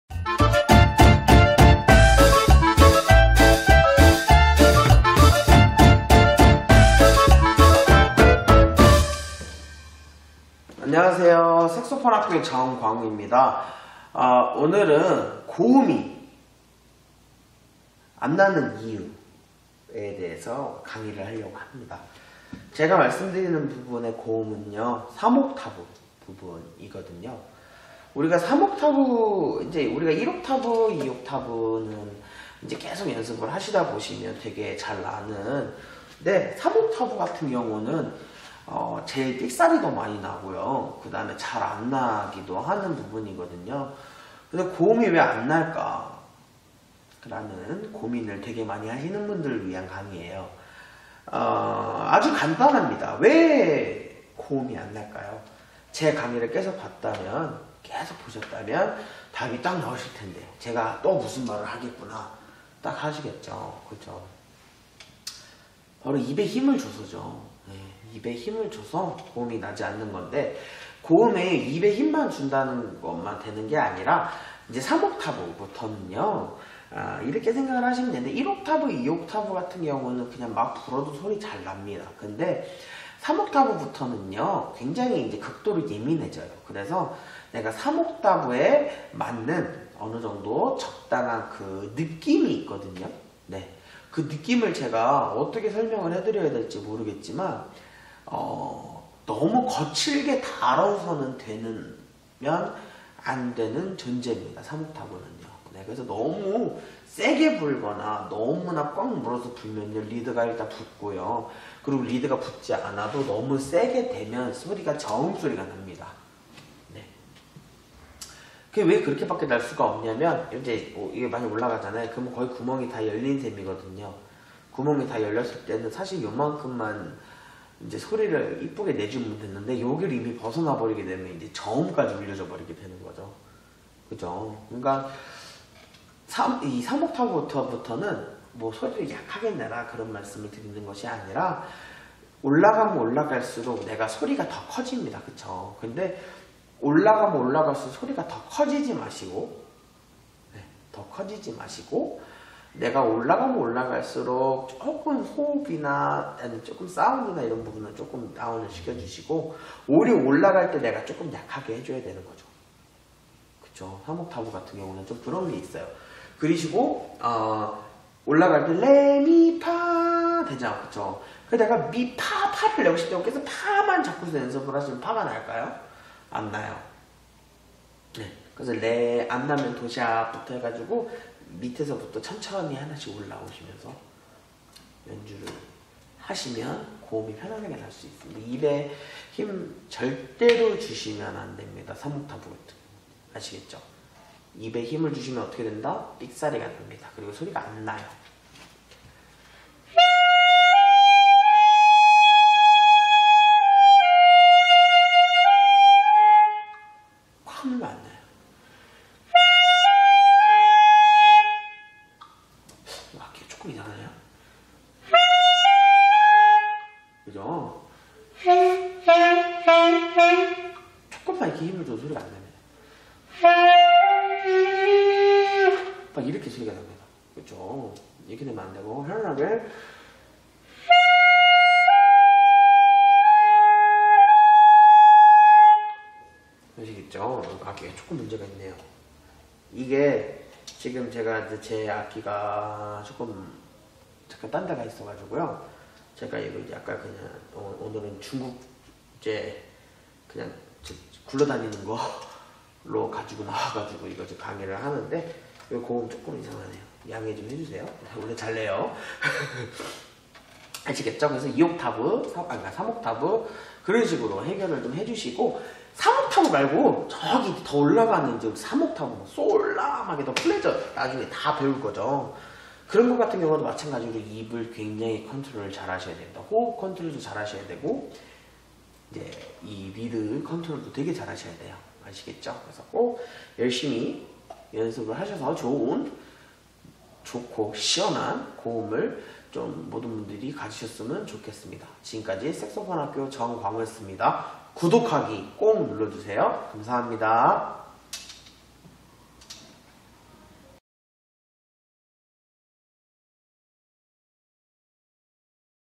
[색소폰학교] 고음이 안나는 이유 (색소폰레슨,색소폰독학,색소폰강의,색소폰배우기,색소폰기초,색소폰동영상강의)
[색소폰학교] 35편

★구독하세요!

오늘은 고음이 안나는 이유에 대해
강의를 했습니다.
질문하고 배울 수 있는 색소폰학교 밴드로 오세요~

악기-셀마액션3 알토색소폰
피스-셀마 컨셉
리가춰-골드스틴
리드-반도린 트레지셔널3호